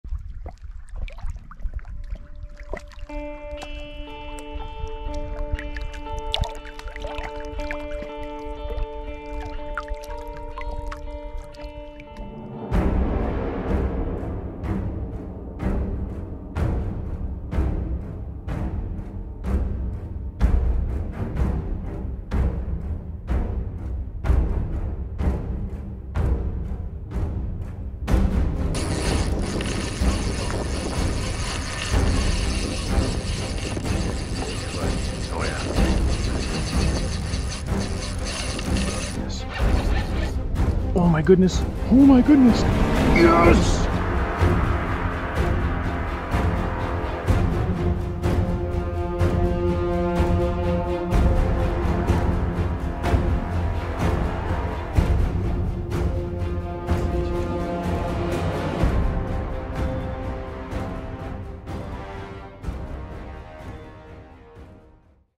Conquering The Yukon
Contouring the Yukon with Wesley Kyle. Join us as we travel the Yukon in search of that next bite.

Intro music @ScottBuckley